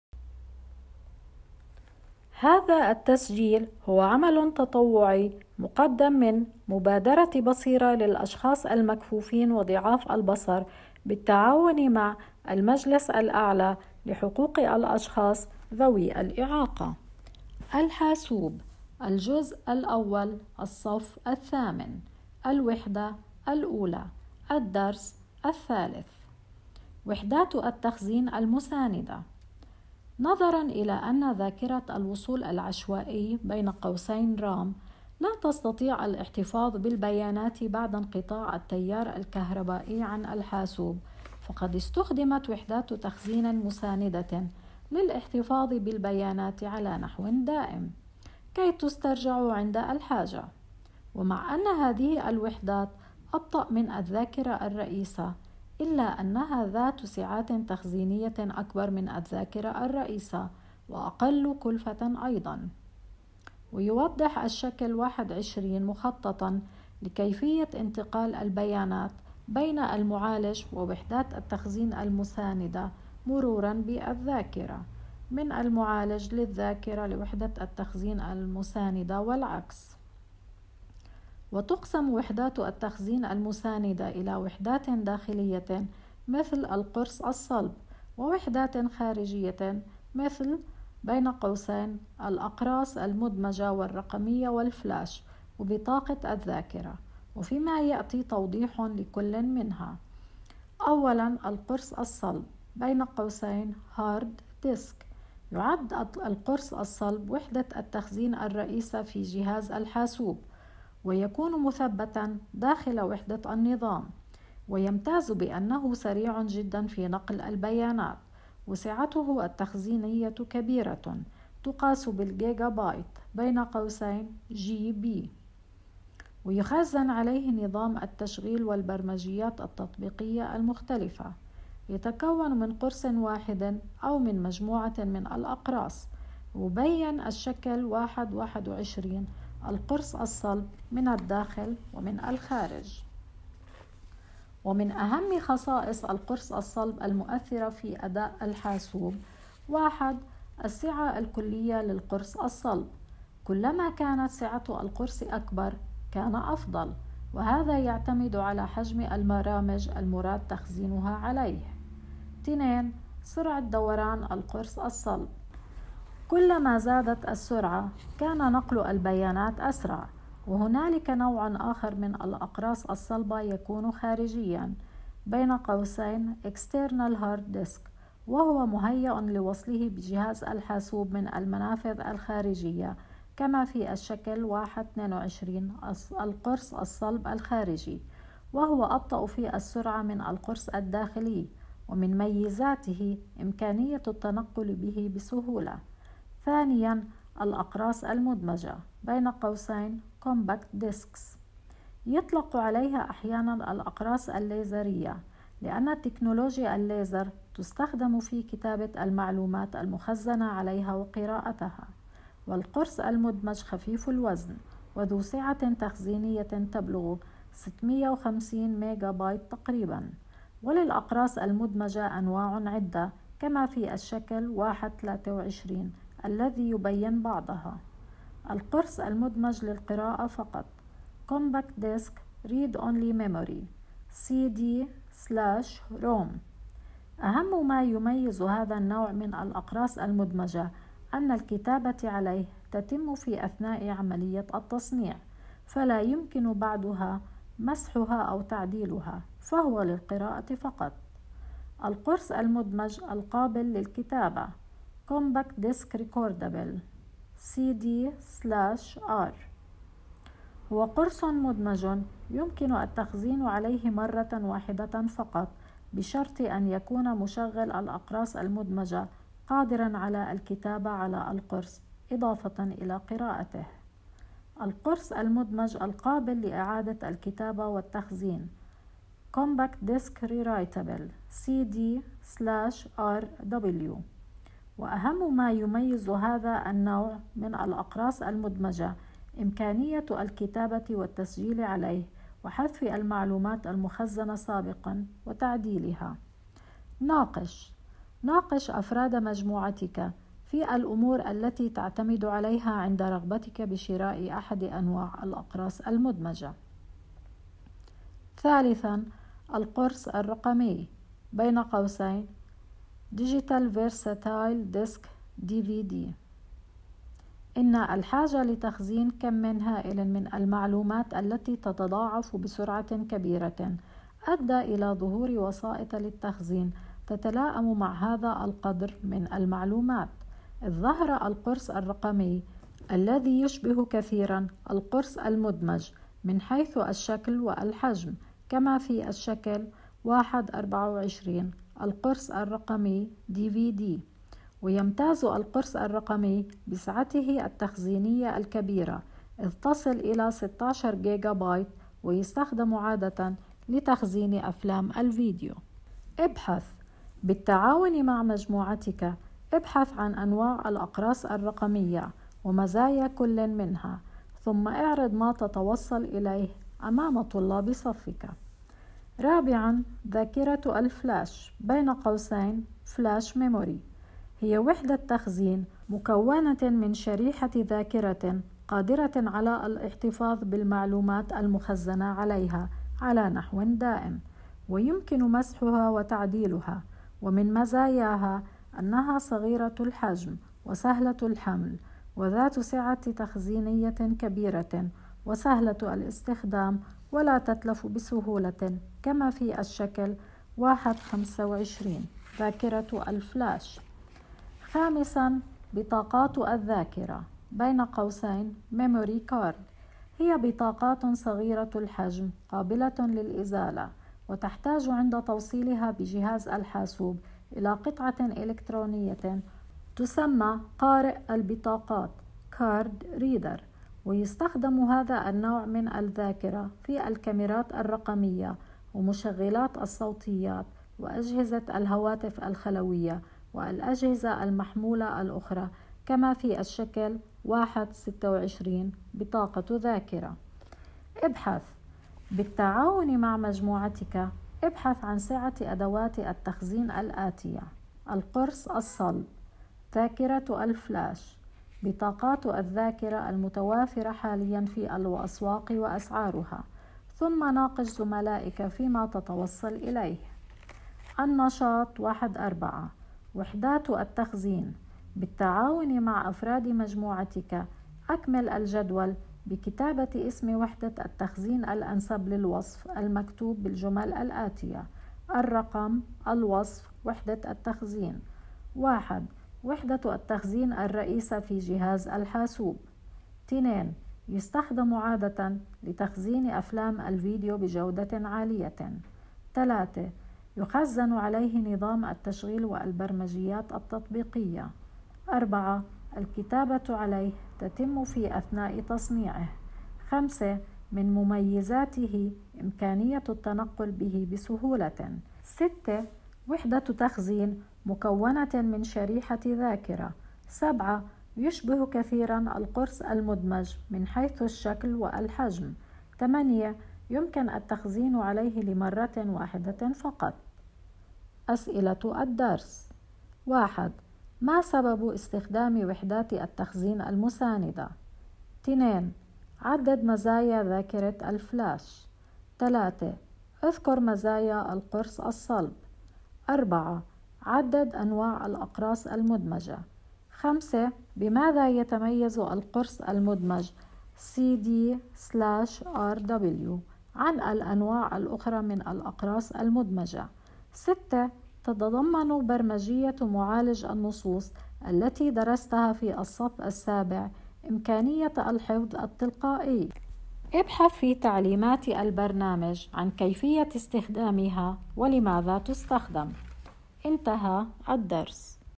الحاسوب | الصف الثامن – الوحدة الأولى – الدرس الثالث
منهاج الصف الثامن للمكفوفين | الصف الثامن – الحاسوب – الوحدة الأولى – الدرس الثاني
تم انجاز هذا التسجيل لصالح المجلس الأعلى لحقوق الأشخاص ذوي الإعاقة ومبادرة بصيرة